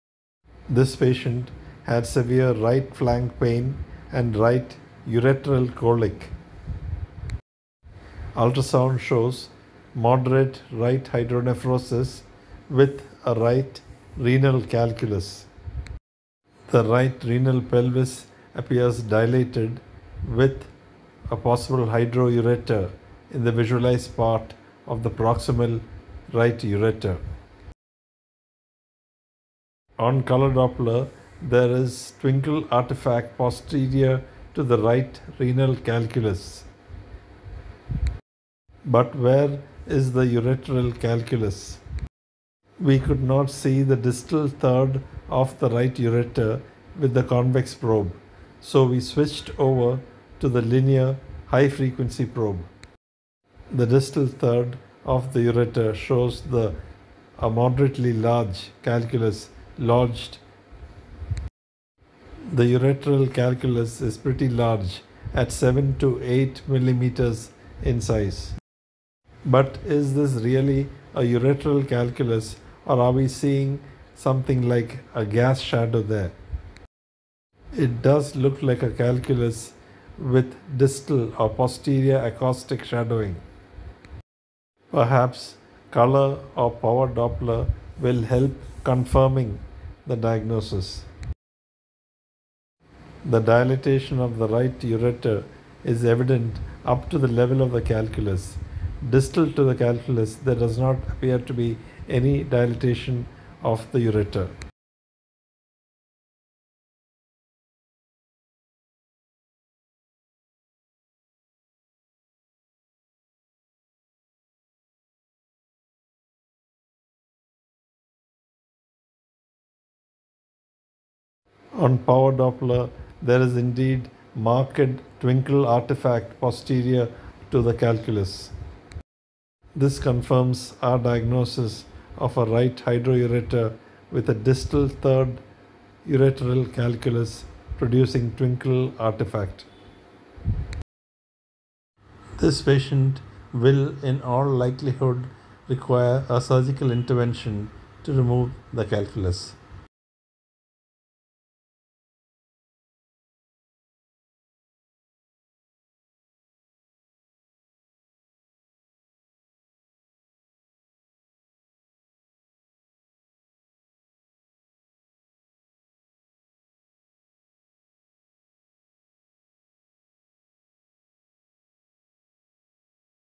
Hydronephrosis with hydroureter, ureteral or ureteric calculus, Ultrasound, color Doppler video
More on sonography of ureteral calculus or ureteric stone disease with hydronephrosis.